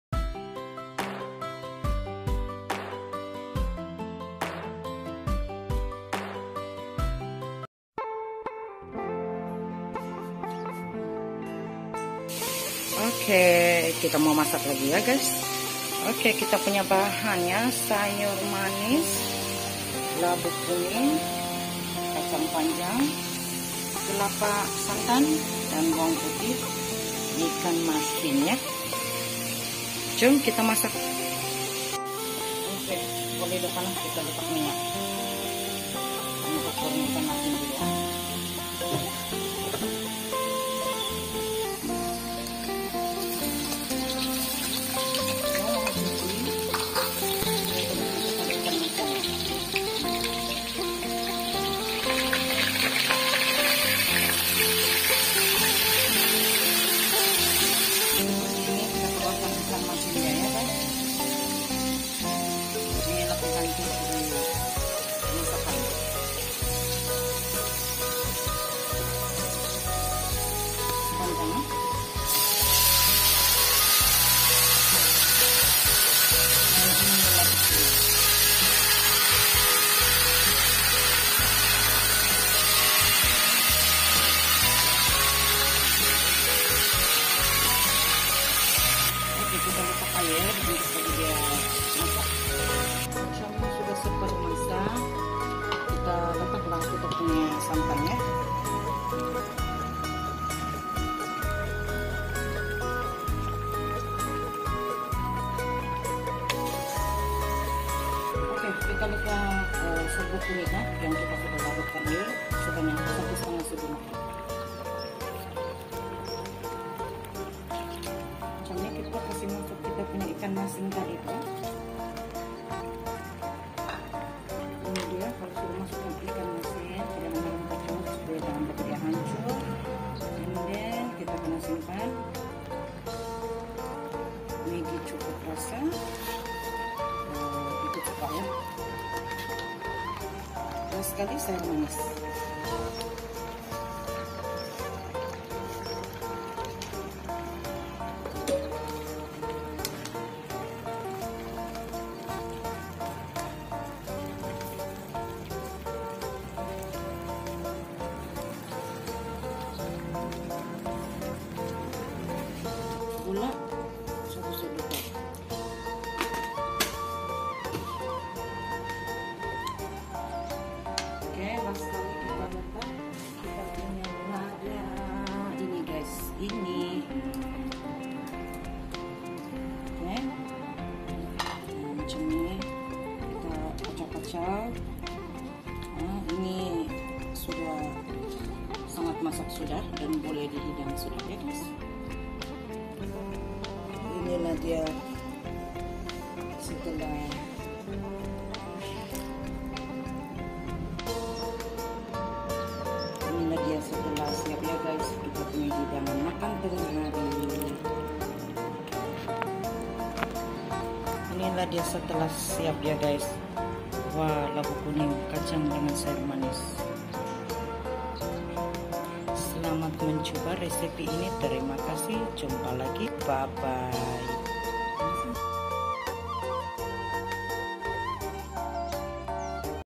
Resepi Labu kuning masak Lemak| simple tapi sedap
hai semua bertemu lagi kita kali ini di menu masakan saya ya..

Labu kuning masak Lemak simple tapi sedap 👍👍

                  bahan-bahan

sayur manis,
labu kuning,
kacang panjang,
santan kotak 200ml
ikan masin,
bawang putih,
serbuk kunyit,
lada besar,
Maggie cukup rasa,
gula pasir,

untuk cara memasaknya sila lah menyaksikan video ini sampai habis ya👍👍

terima kasih kawan semua kerana sentiasa support channel ini jangan lupa tinggalkan komen anda supaya saya dapat melawat channel anda terima kasih semua 👍👍